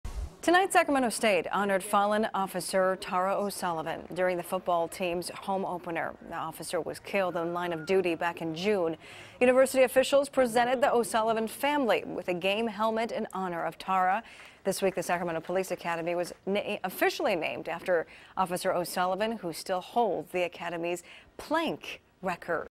Officer Tara O'Sullivan Honored At Sac State Football Home Opener
Sacramento State honored the fallen officer tonight in their home opener.